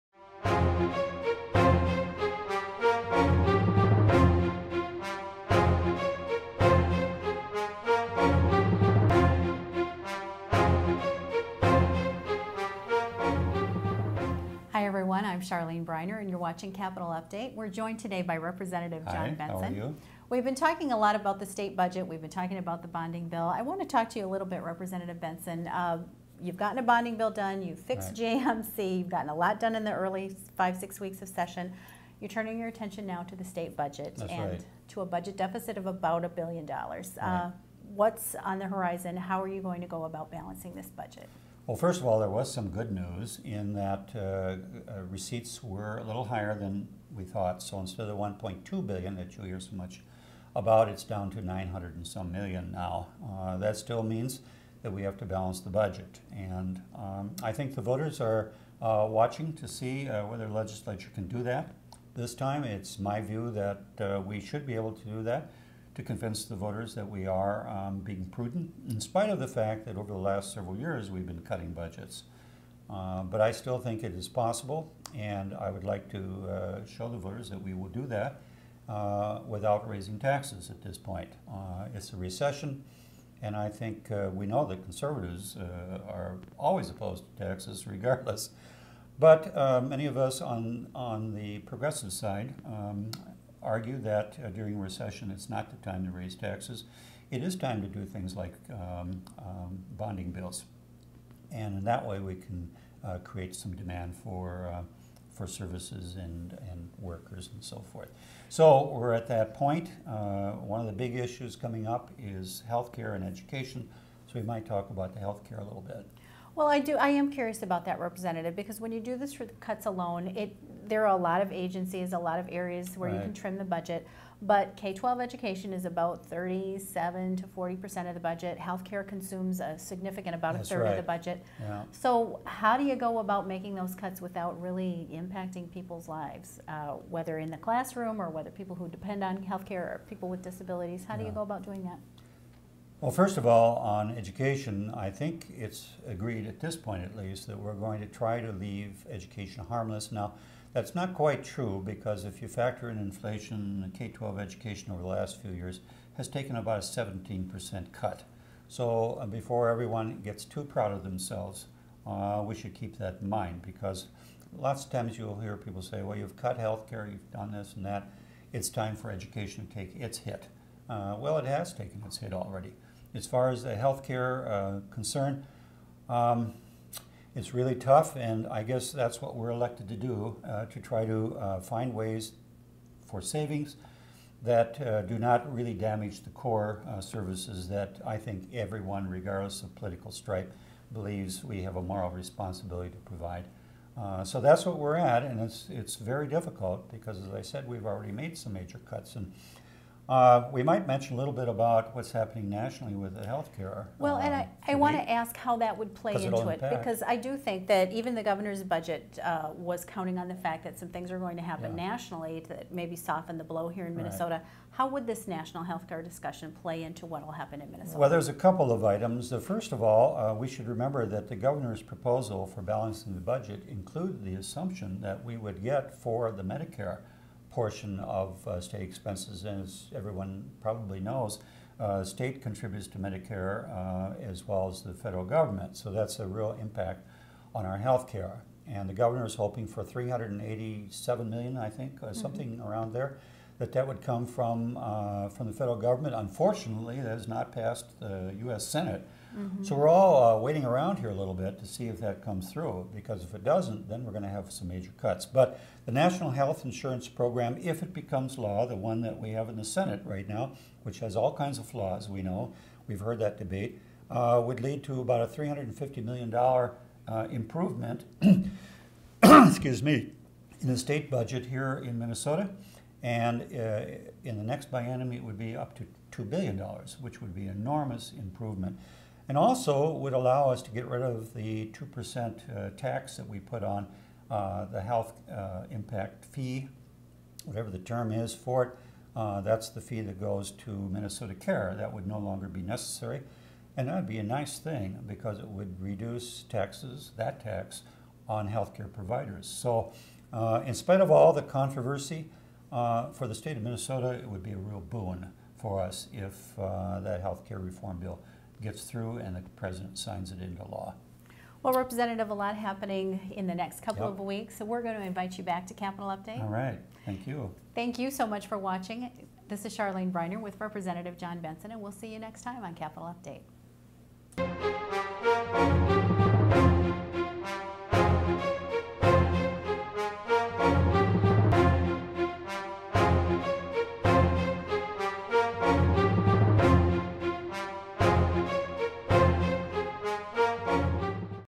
Rep. John Benson on the State Budget
Rep. John Budget discusses the State Budget and the challenge of solving a nearly $1 billion deficit.